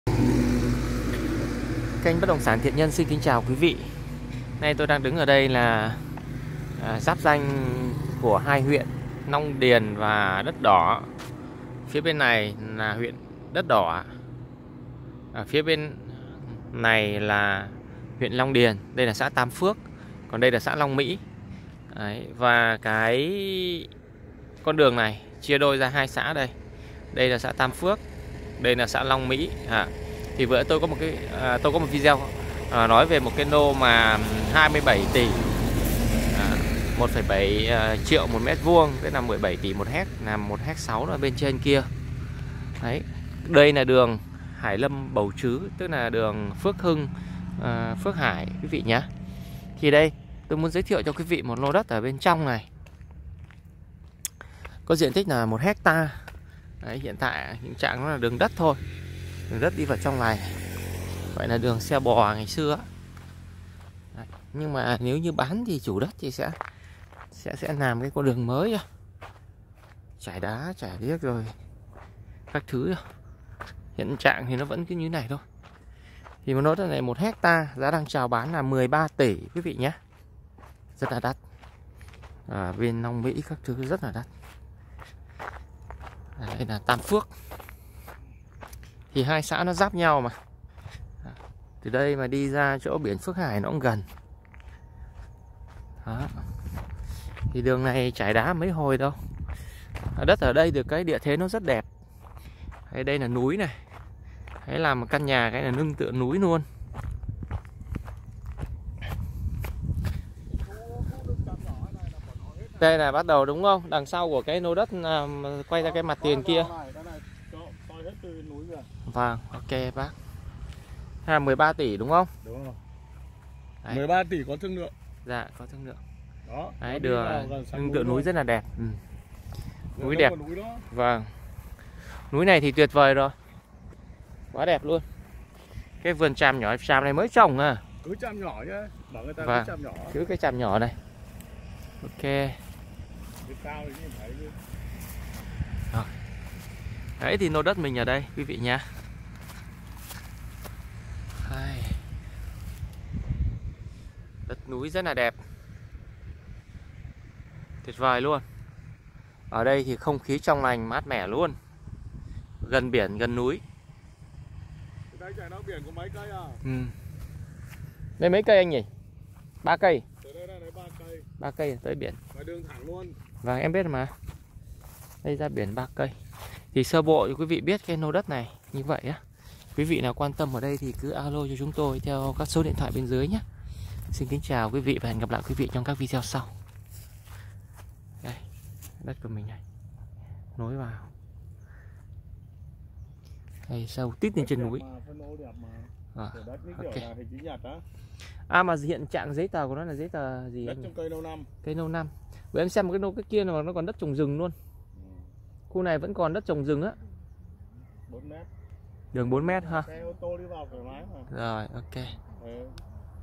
NVT1580 Chỉ 13 tỷ cho 1 hecta đất ngay núi Minh Đạm, gần biển Phước Hải
NVT1580 Bán lô đất đẹp giáp danh giữa Long Điền, Đất Đỏ, Bà Rịa - Vũng Tàu.
- Diện tích: 1 ha. Giá bán: 13 tỷ
- Tiện ích: Cách biển Phước Hải 3km, trên đất đang trồng tràm, lô đất đẹp lưng tựa núi, gần khu di tích lịch sử núi Minh Đạm, rất thích hợp đầu tư phân lô, làm khu nghỉ dưỡng.

Trân trọng!!!
Mr Đoàn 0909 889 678 (Viber/Zalo)/ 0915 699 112 (Viber/Zalo)
Mrs Quỳnh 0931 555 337